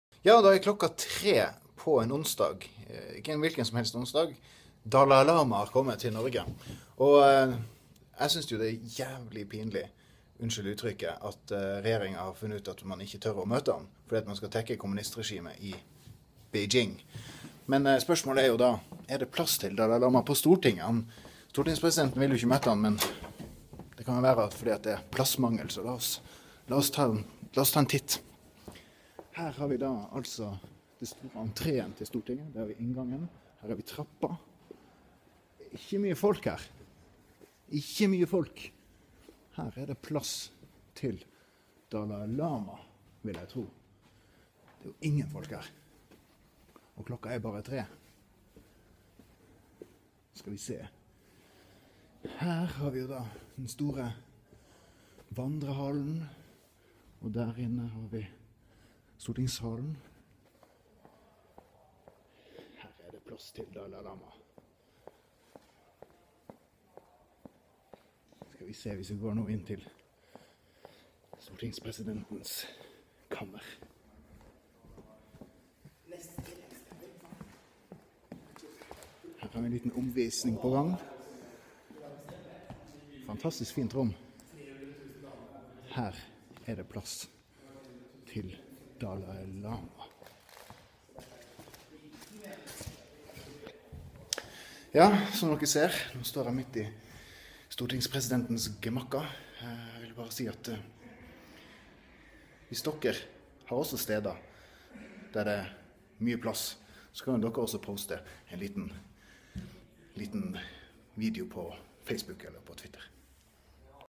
hererdetplasstilDalaiLama
Er det slik at det ikke er plass til Dalai Lama i kongeriket Norge?